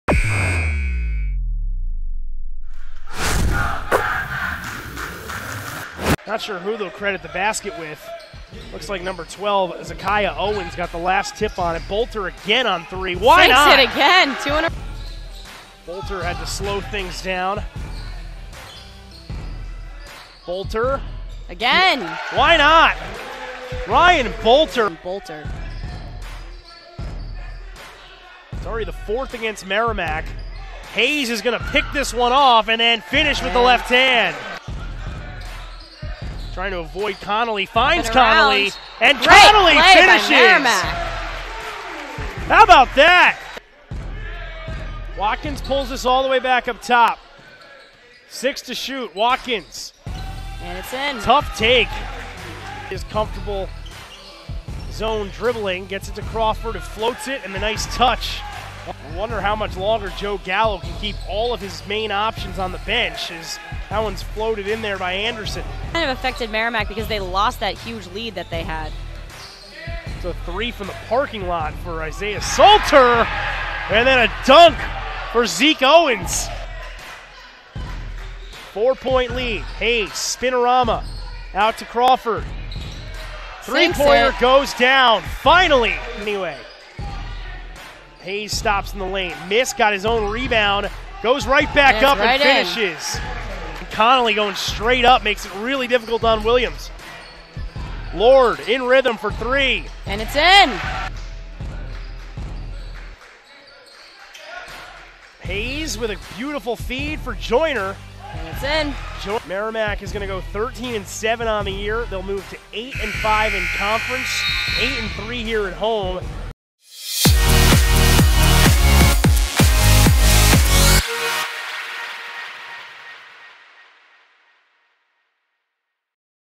MBB: Highlights vs AIC (012919)
The Merrimack men's basketball program improved to 8-2 over its last 10 games with a 74-61 victory over American International on Tuesday evening at Hammel Court!